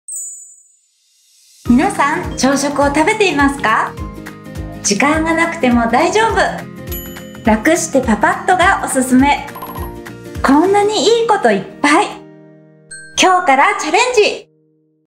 超ラク！朝めしチャレンジ（告知あり）
名古屋市では、健全な食生活の実践に向けて食育の推進に取り組んでいますが、特に20代から30代の若い世代における朝食摂取率が低い傾向にあります。
そこで、朝が苦手、時間がない、そんな人にもおすすめの「アイデア時短朝食レシピ」をInstagramもしくはTwitterで募集し、若い世代に頑張らずに朝食をもっと美味しくとってもらえるようレシピコンテストを開催するとともに、「超ラク！朝めしチャレンジ」として動画を作成しました。
〈2021年8月制作　健康福祉局健康増進課〉